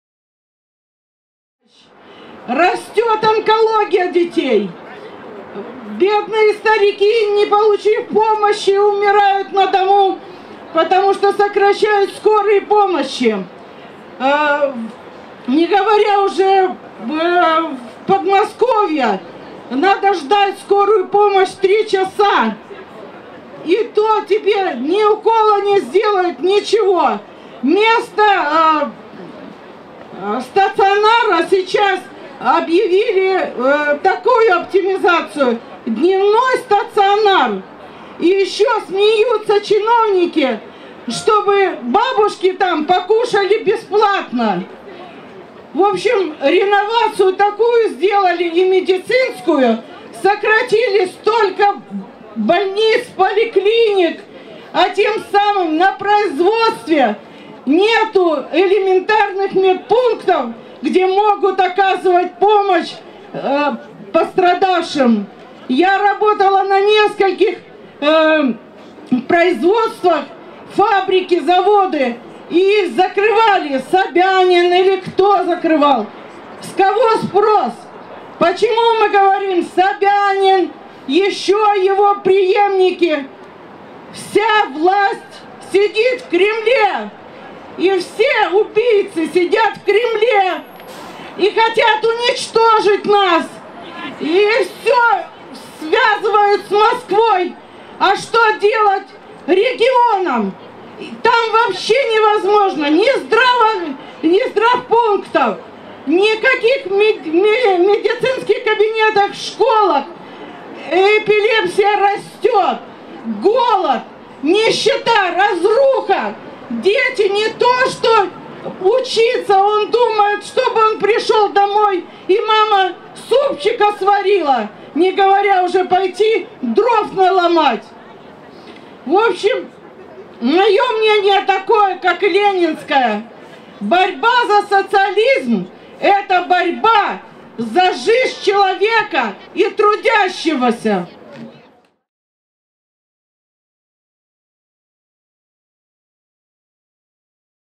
Женщина на митинге в Москве: «Убийцы сидят в Кремле!»
Поддержать финансами проект «Рупор Москвы»:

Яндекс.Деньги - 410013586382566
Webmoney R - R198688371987
Bitcoin - 1AJJCS1D7bqnrDiqCX6BTC8NmbqE8BE7Ha
Bitcoin Cash - qqatxpdu3k89gjyjj2808z73d9hcy879c54h4pkqya
Ether - 0xE358EC6f52B89be1596B9E35e39C6bdF61e274D3